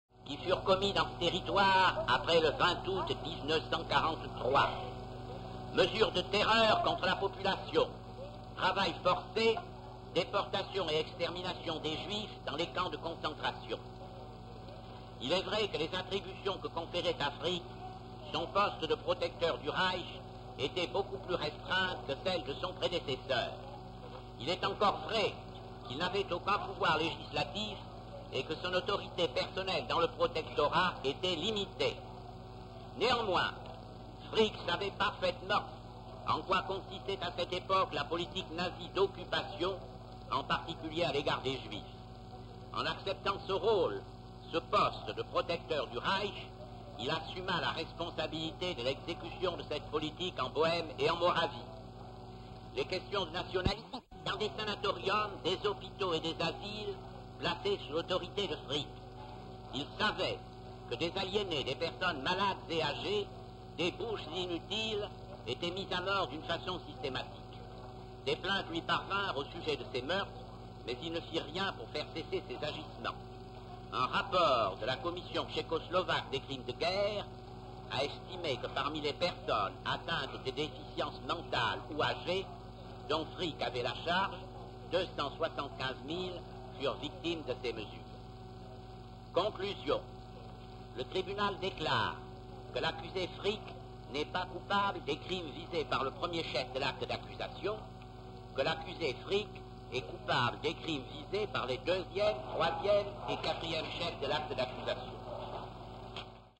Nuremberg Day 218 Frick Judgment
On Oct. 1, 1946, Wilhelm Frick, Reich Minister of the Interior from Jan. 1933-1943 and thereafter Reich Protector of Bohemia and Moravia, was found guilty on three counts and sentenced to death by hanging.